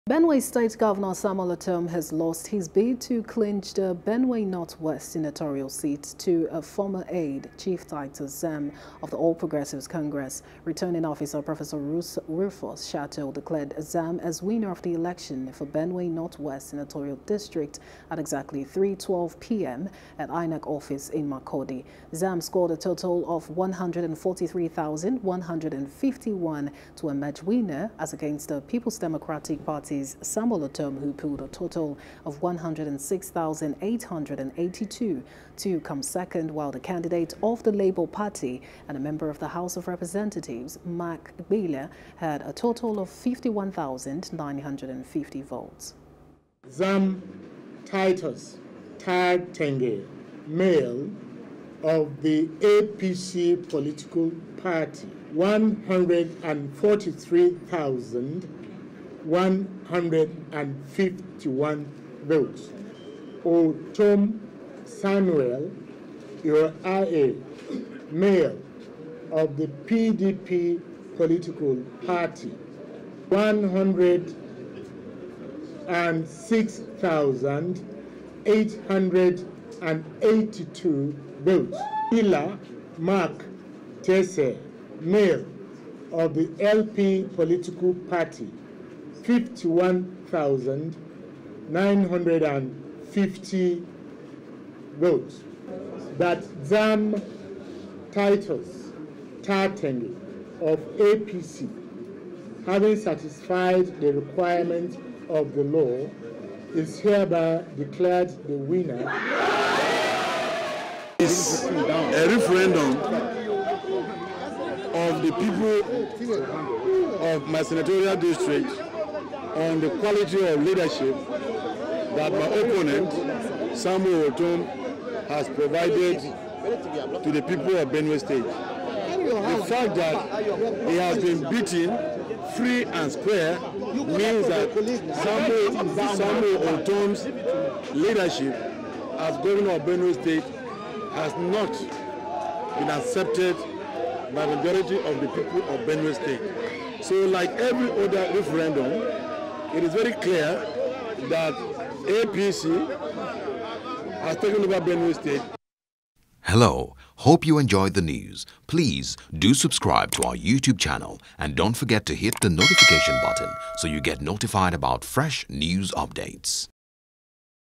General Elections: Ortom Loses Senatorial Bid To Ex Aide
Benue State Governor, Samuel Ortom has lost his bid to clinch the Benue North West. senatorial seat to a former aide, Chief Titus Zam of All Progressives Congress (APC).